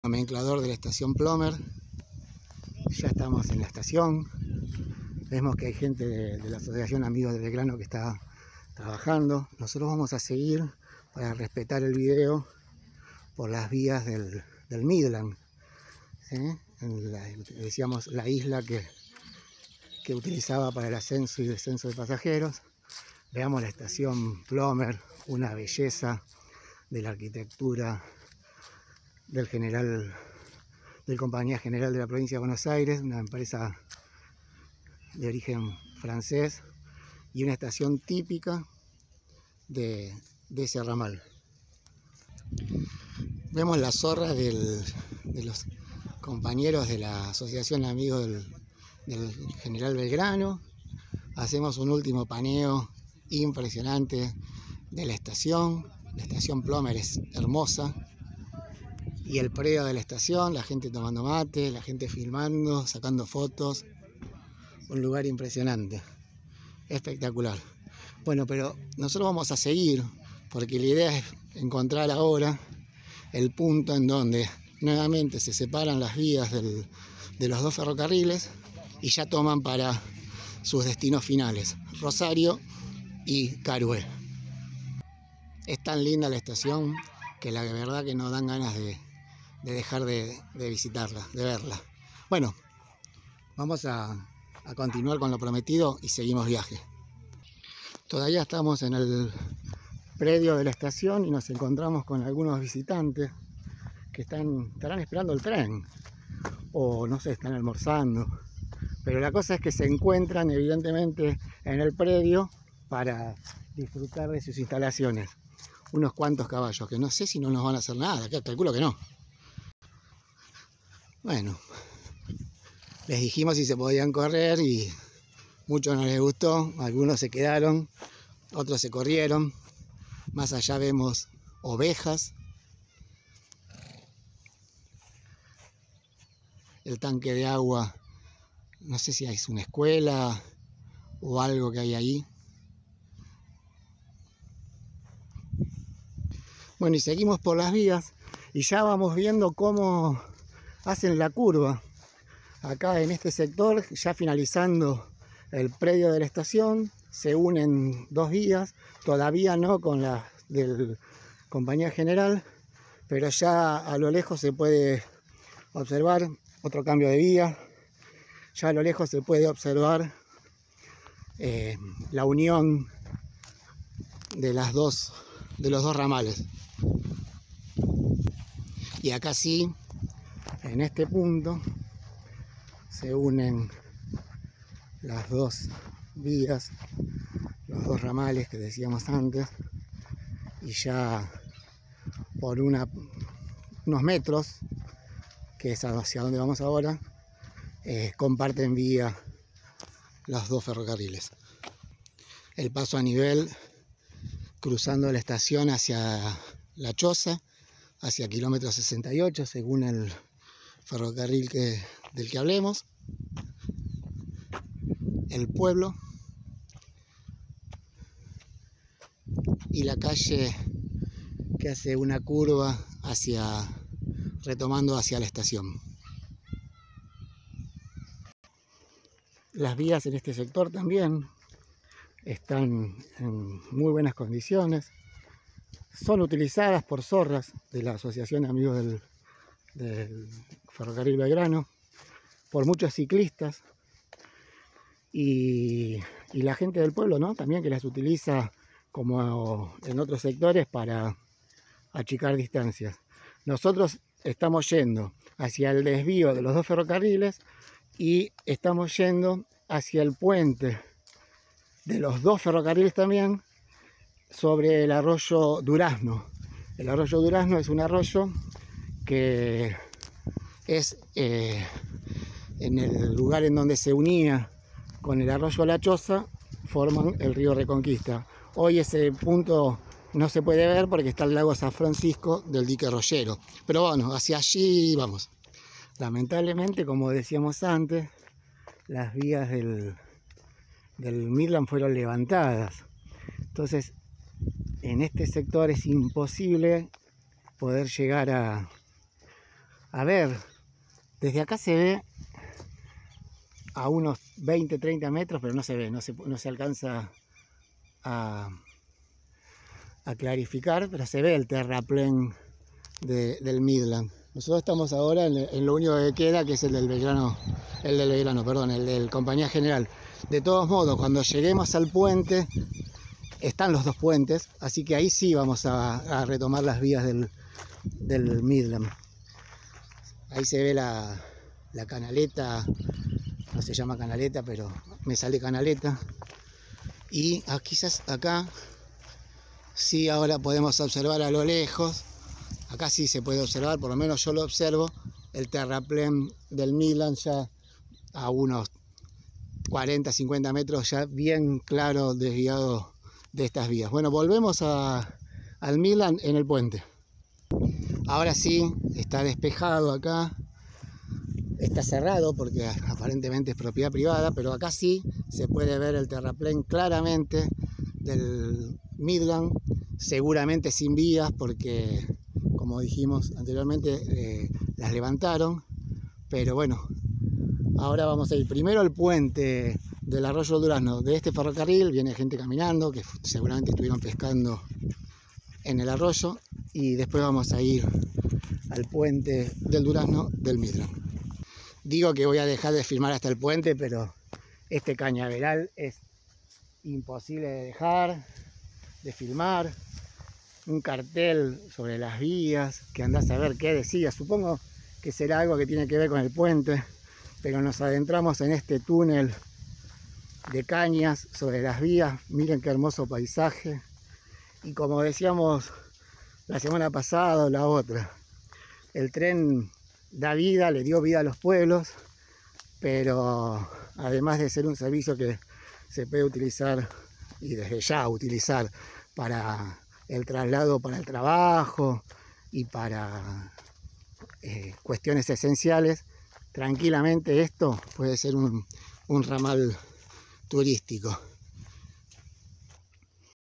Por las vías del MIDLAND en PLOMER (2da parte, predio de estación y tunel vegetal en vías del CGBA)
Segunda parte de la visita a PLOMER recorriendo la traza del MIDLAND y del CGBA en este caso.
Visitamos el predio ferroviario de la estación y su impresionante infraestructura, la hermosa estación y luego continuamos caminando hacia la bifurcación de ambos ramales y el arroyo El Durazno.
Vamos a observar cómo a partir de dejar este sector las vías del MIDLAND ya no existen debido a que fueron levantadas en 1980.
Nos sorprendió un espectacular "Tunel vegetal" sobre las vías del CGBA, de cañas, en gran parte del trayecto que hicieron de la caminata una experiencia visual incomparable.

18 DE ABRIL DE 2021